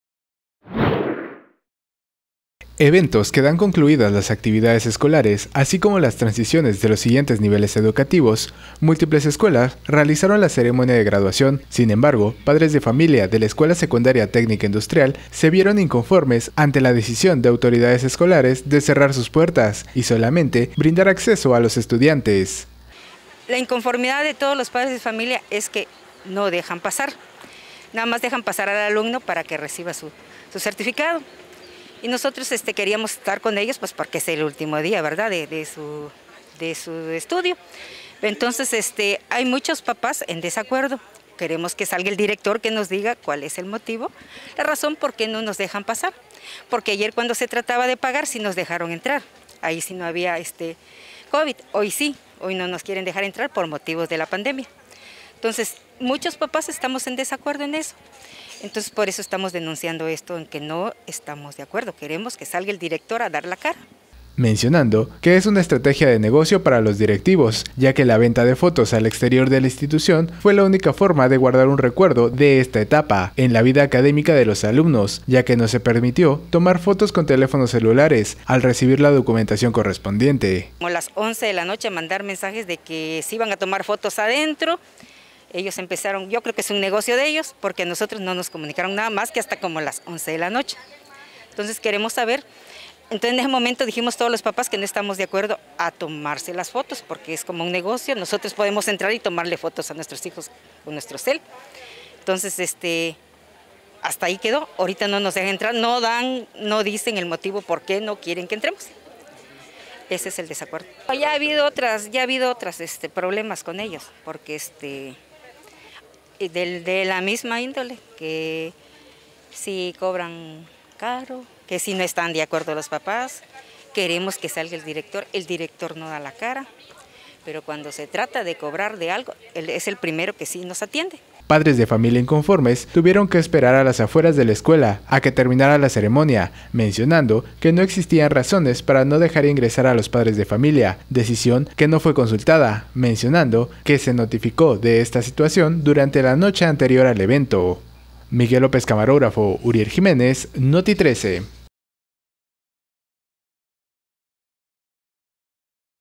Padres de familia inconformes al no poder ingresar a la graduación al interior de escuela| NOTITRECE
Padres de familia de la escuela secundaria técnica industrial se vieron inconformes ante la sesión de autoridades escolares de cerrar sus puertas y solamente brindar acceso a los estudiantes.

✍🏻 Uriel Jiménez /